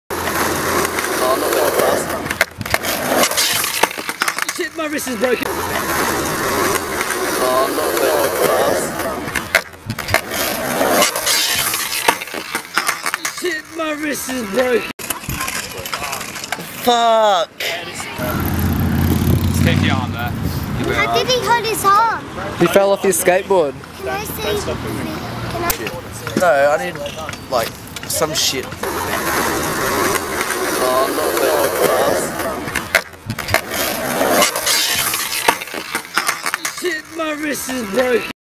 Alec is Unlucky........
Alec Chapman Brakes his wrist :(